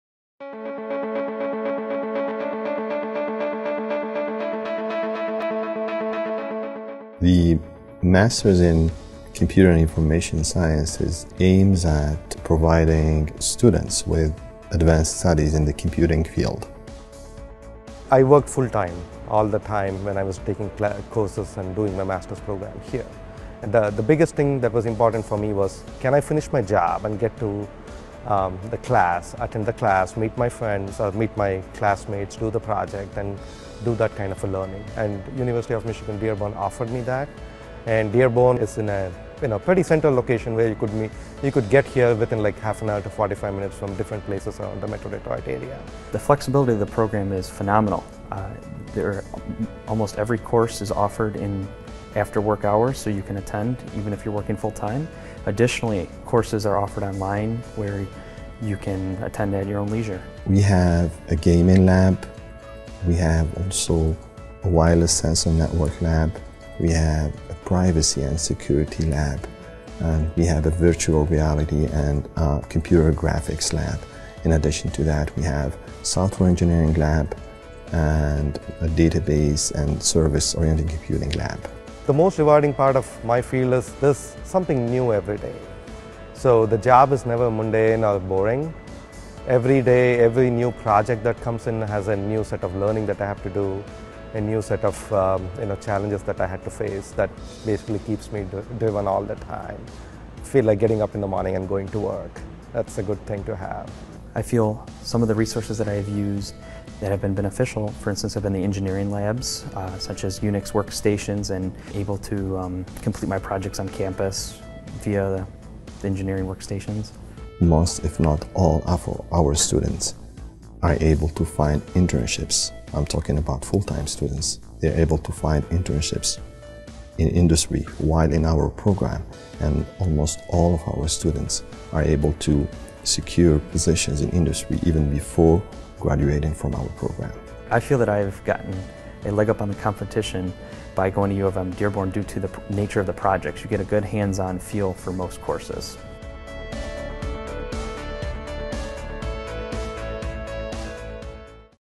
UM-Dearborn MS in Computer and Information Science
The MS in Computer and Information Science provides advanced studies in computing fields with emphasis on networking, security and privacy, computer graphics, game design, software engineering, data management, information systems, web technologies, mobile and pervasive computing and systems software.